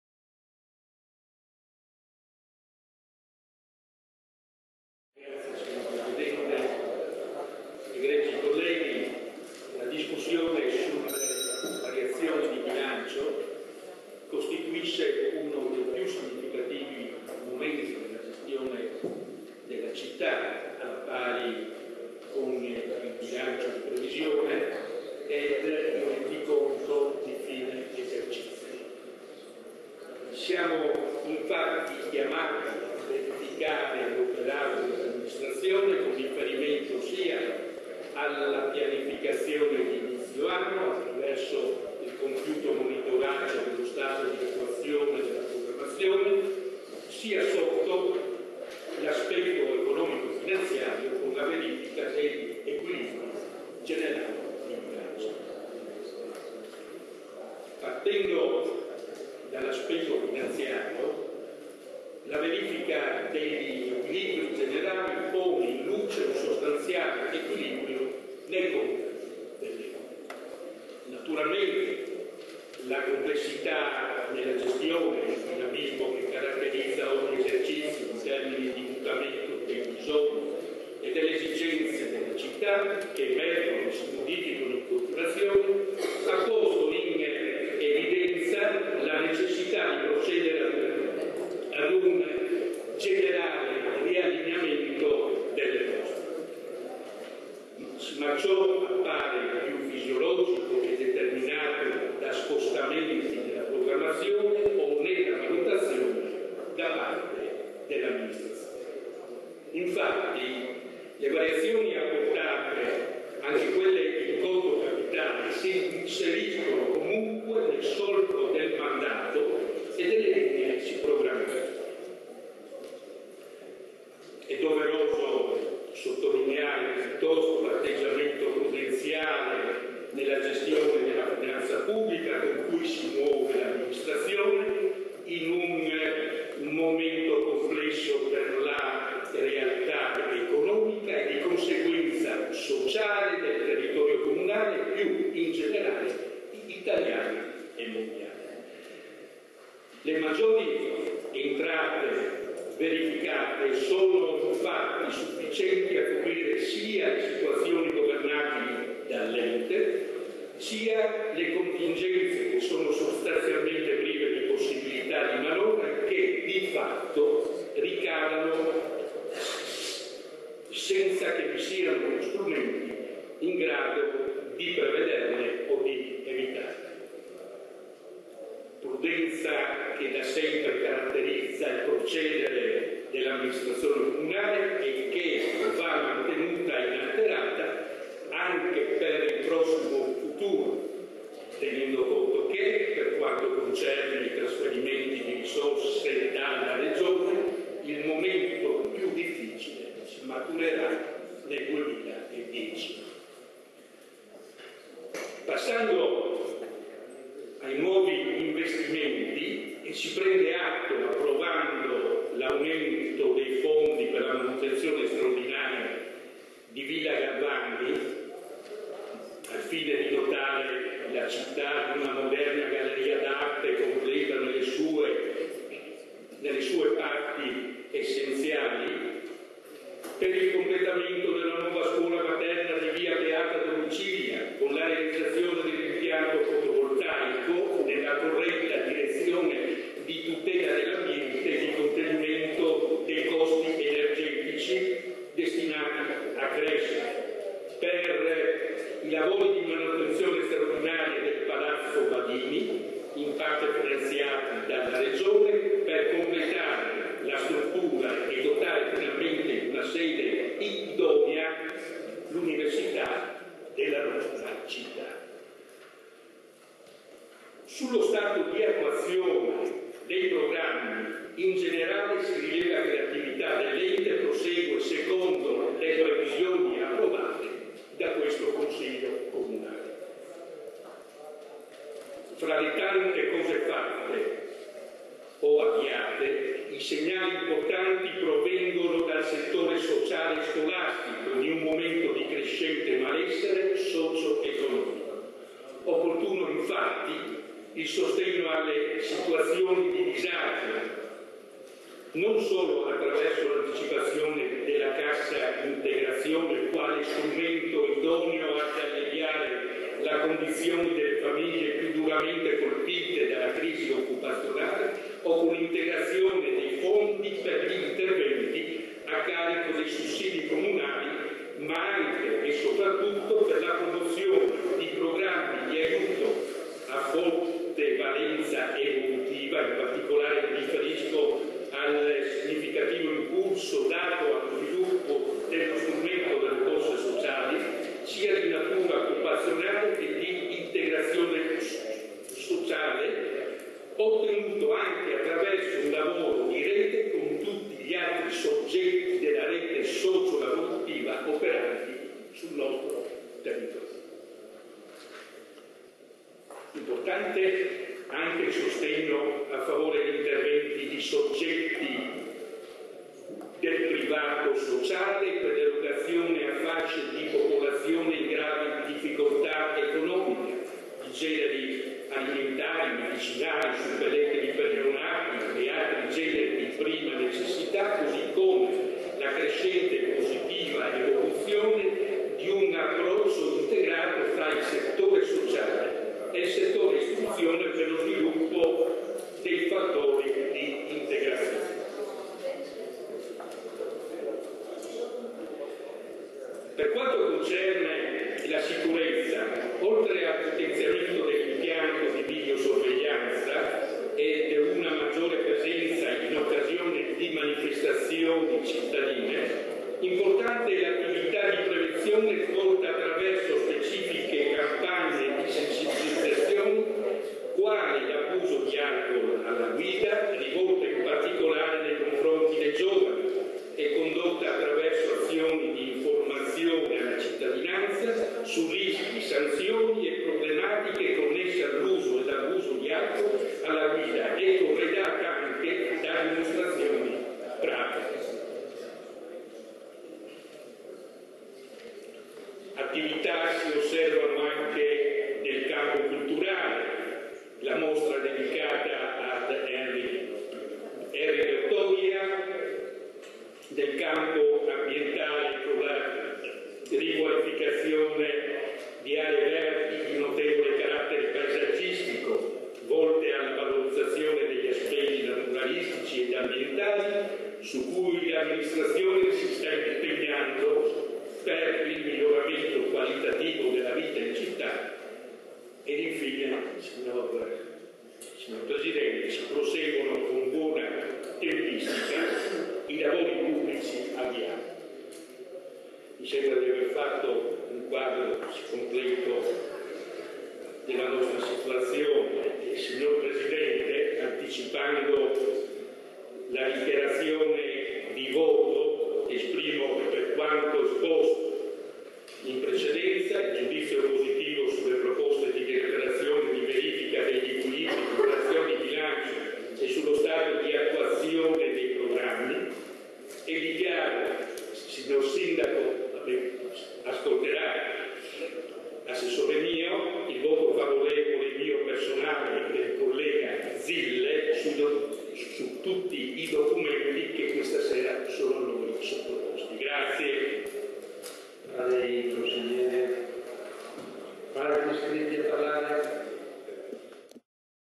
Variazioni di bilancio
Replica del Consigliere Comunale Averna alle delibere 46, 47, 48, 49: variazioni di bilancio.